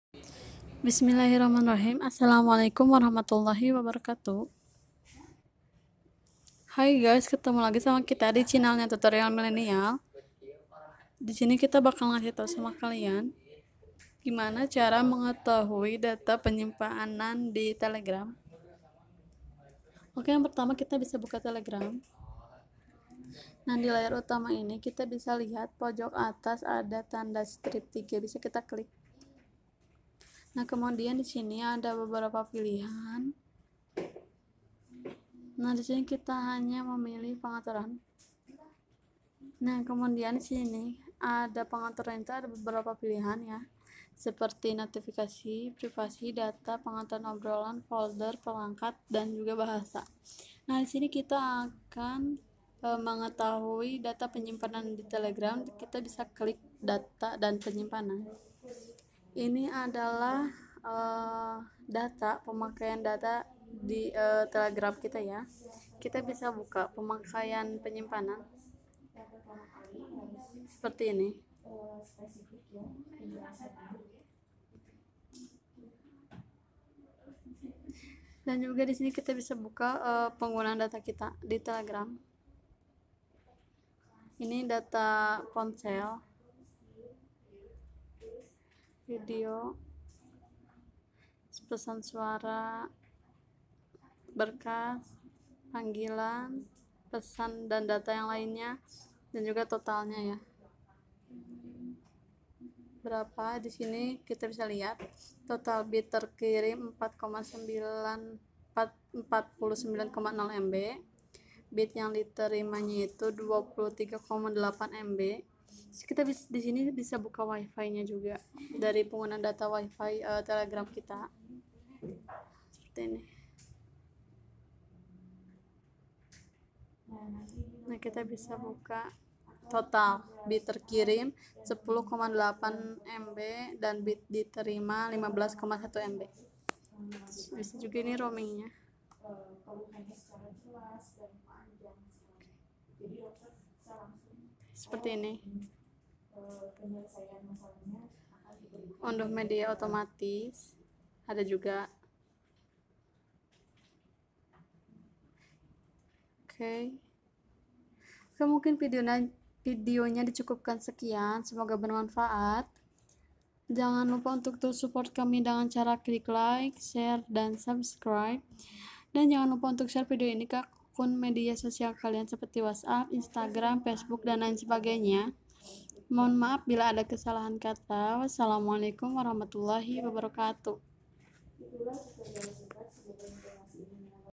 Cara mengetahui Data Penyimpanan di Telegram
tips dan cara mudah  mengetahui penyimpanan data seluler di Telegram